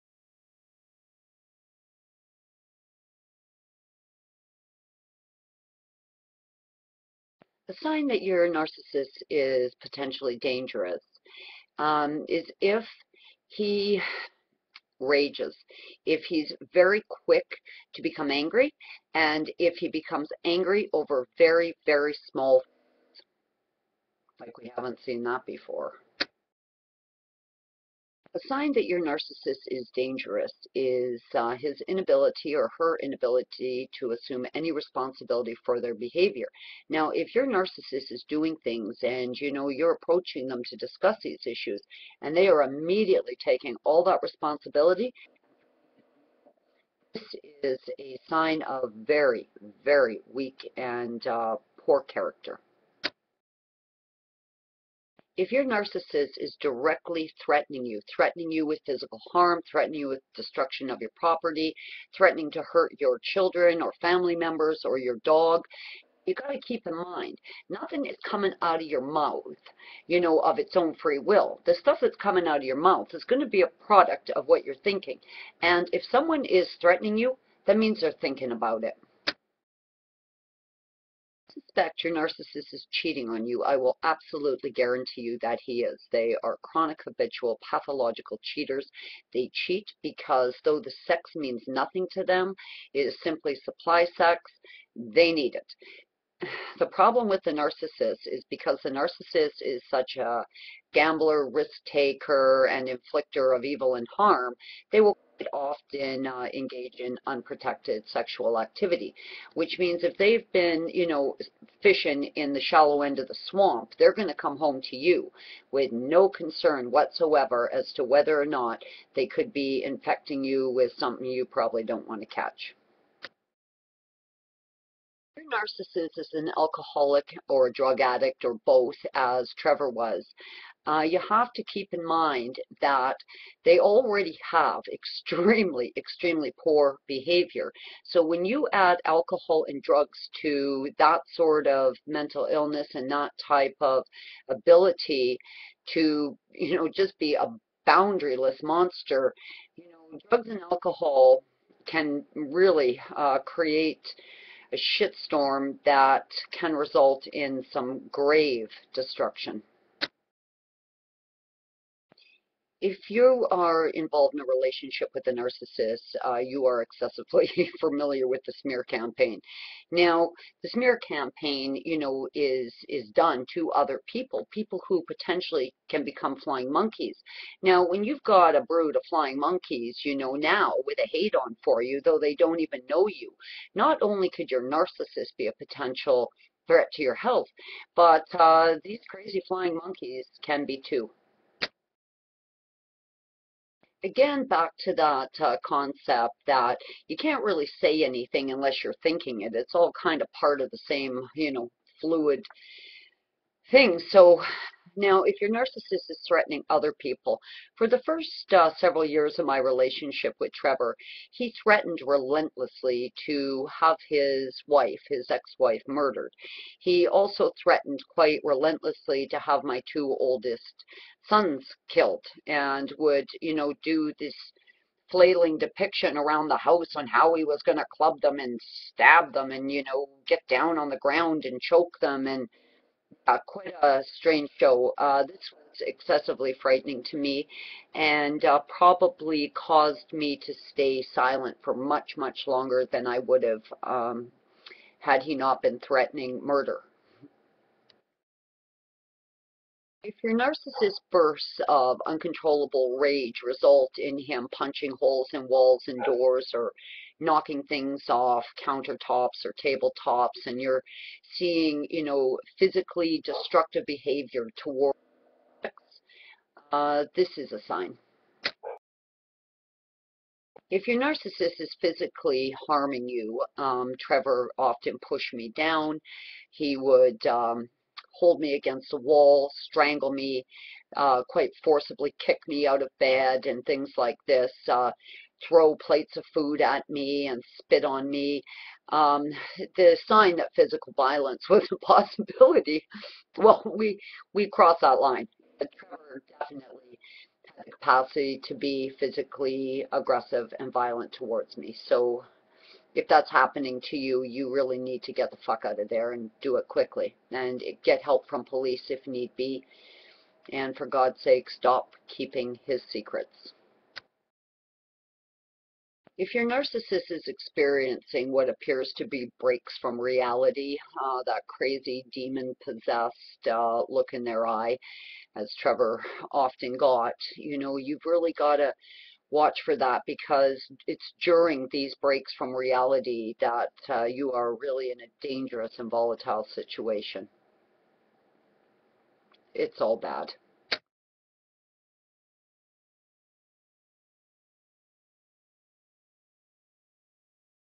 10 Signs A Narcissist Is Dangerous
No group of people are more likely to inflict grave harm or commit murder. If you are involved with a narcissist you are likely in serious danger.